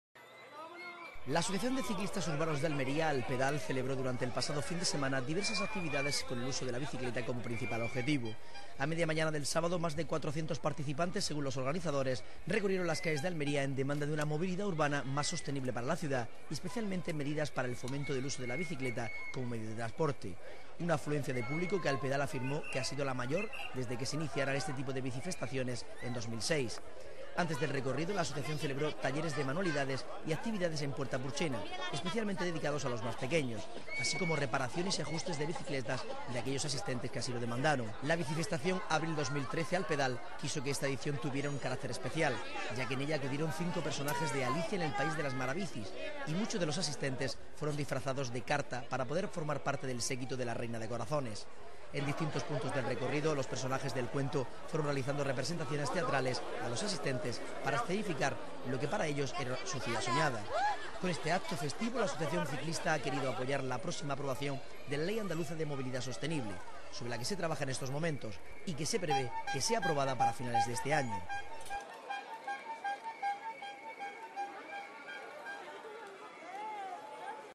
La Bicifestación de Alpedal defendió una movilidad más sostenible
La asociación de ciclistas urbanos de Almería, Alpedal, celebró durante el pasado fin de semana diversas actividades con el uso de la bicicleta como principal objetivo. A media mañana del sábado, más de 400 participantes, según los organizadores, recorrieron las calles de Almería en demanda de una movilidad urbana más sostenible para la ciudad y especialmente medidas para el fomento del uso de la bicicleta como medio de transporte. Una afluencia de público que Alpedal afirmó que ha sido la mayor desde que se iniciaran este tipo de Bicifestaciones en 2006. Antes del recorrido la asociación celebró talleres de manualidades y actividades en Puerta Purchena, especialmente dedicados a los más pequeños, así como reparaciones y ajustes de bicicletas de aquellos asistentes que así lo demandaron.  El recorrido partió a las 11.30 circulando, como ya es habitual, por el Paseo de Almería, Avenida de Cabo de Gata, Avenida del Mediterráneo, Padre Méndez, Carretera de Ronda, Gregorio Marañón y Avda. Federico García Lorca, para finalizar de nuevo en la Plaza de las Velas. La Bicifestacion abril2013 Alpedal quiso que esta edición tuviera un carácter especial, ya que a ella acudieron cinco personajes de 'Alicia en el país de las marabicis' y muchos de los asistentes fueron disfrazados de carta para poder formar parte del séquito de la Reina de Corazones. En distintos puntos del recorrido los personajes del cuento fueron realizando representaciones teatrales a los asistentes para escenificar la que para ellos era su ciudad soñada. Con este acto festivo la asociación ciclista ha querido apoyar la próxima aprobación de la Ley Andaluza de Movilidad Sostenible, sobre la que se trabaja en estos momentos y que se prevé que sea aprobada para finales de año.